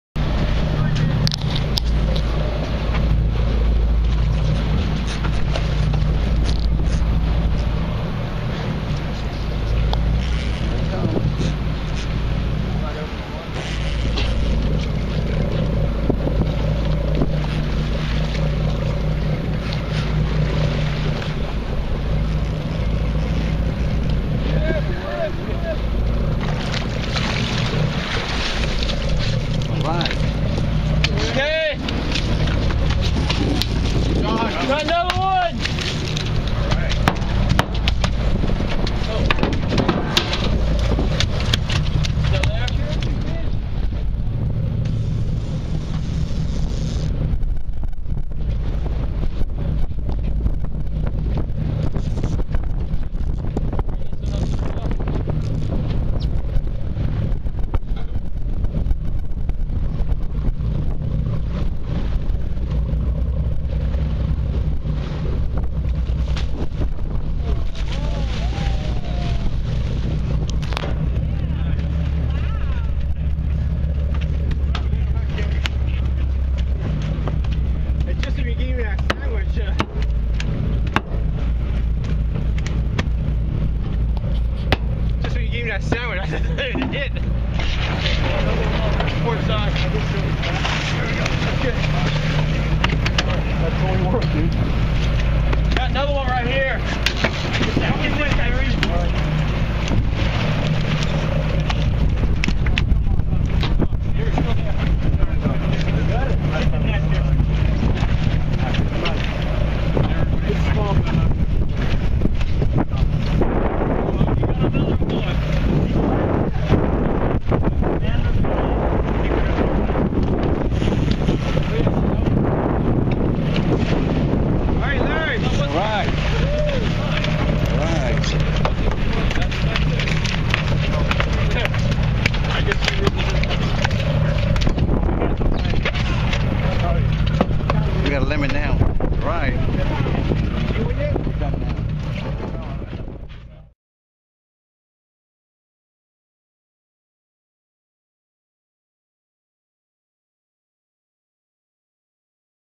Salmon Trolling on the Hog Heaven 06/30/2017 [HD]
18 limits to 20 pounds.